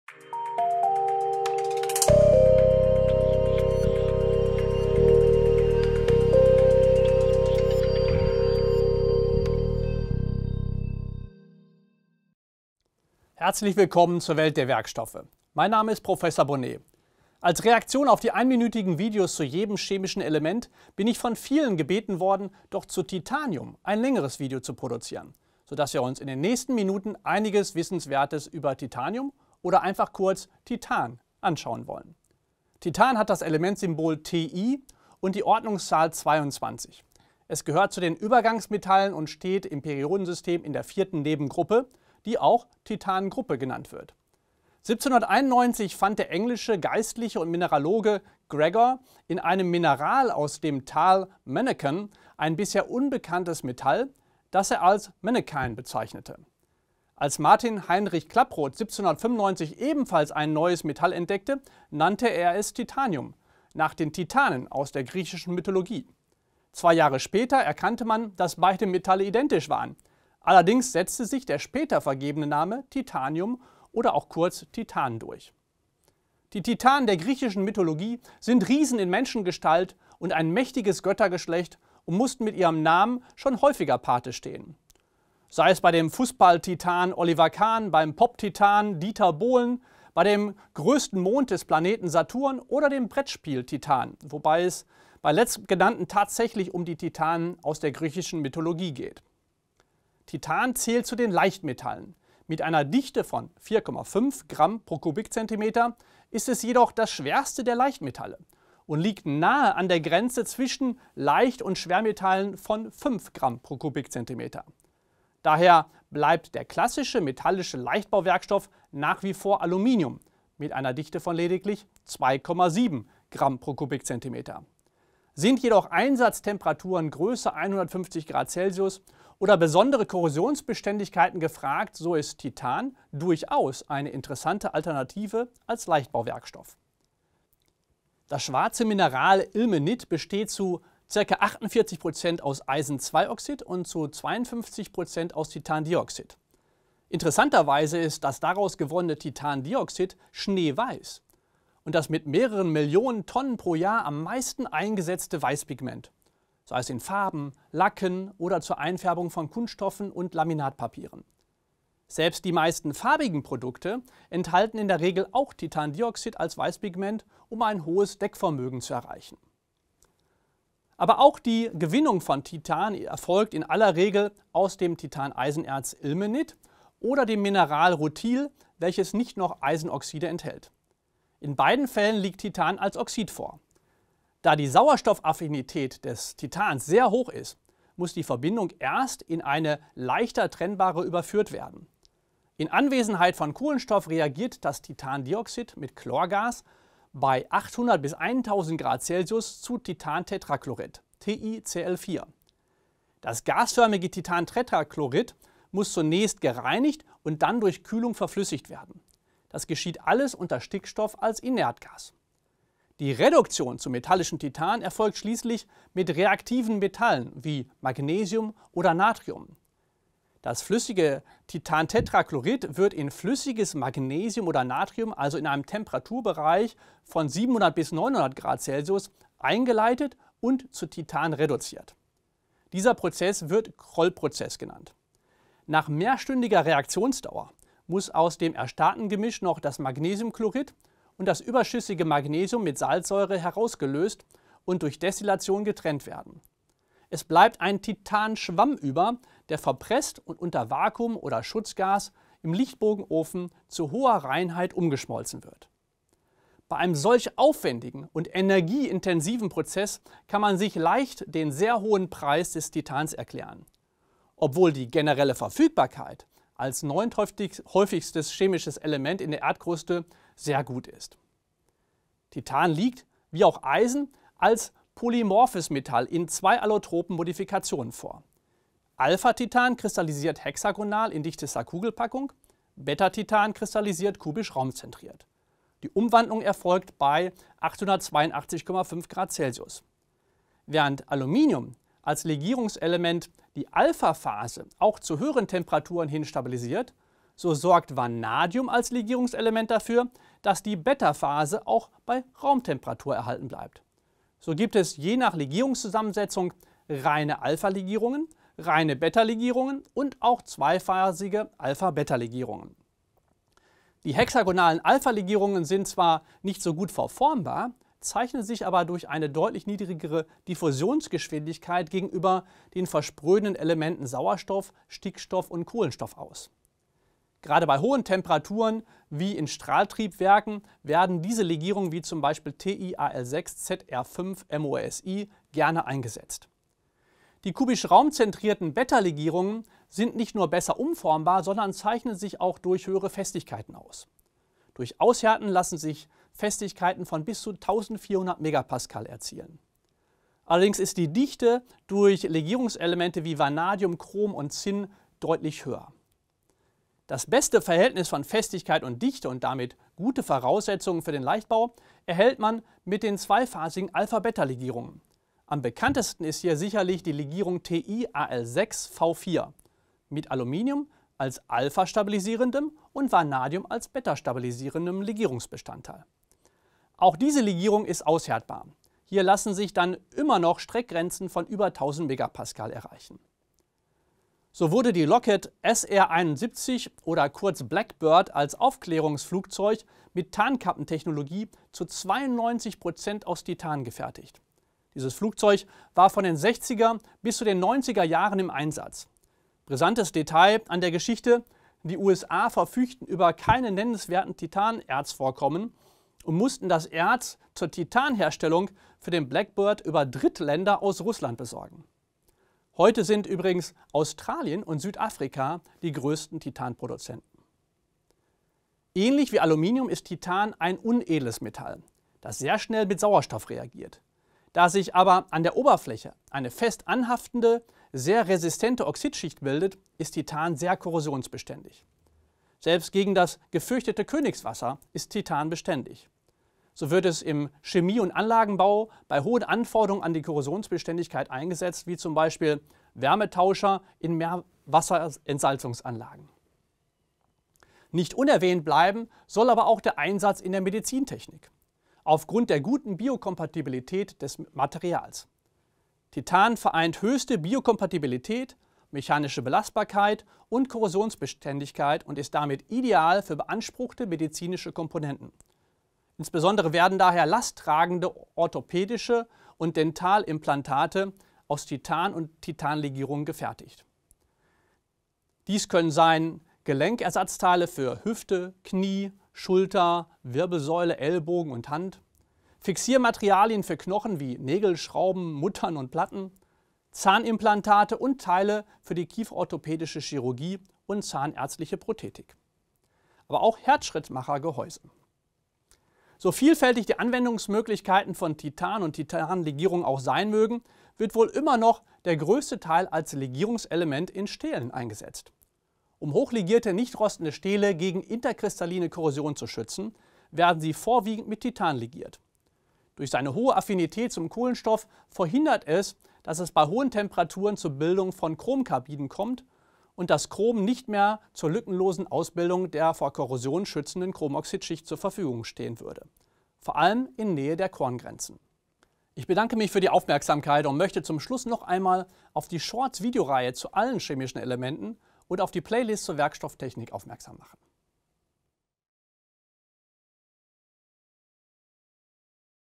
Titanium Das Video
Alles Wissenswertes zum Thema Titanium in weniger als 10 Minuten
00:00 - Einleitung
00:32 - Historisches
01:16 - Titanen
01:40 - Leichtmetall Titan
02:17 - Titandioxid
02:50 - Kostenintensive Gewinnung
04:32 - Alpha- und Beta-Titan
06:24 - Luft- und Raumfahrttechnik
06:58 - Korrosionsbeständigkeit
07:27 - Einsatz in der Medizintechnik
08:15 - Legierungselement in Stählen
08:48 - Schlussworte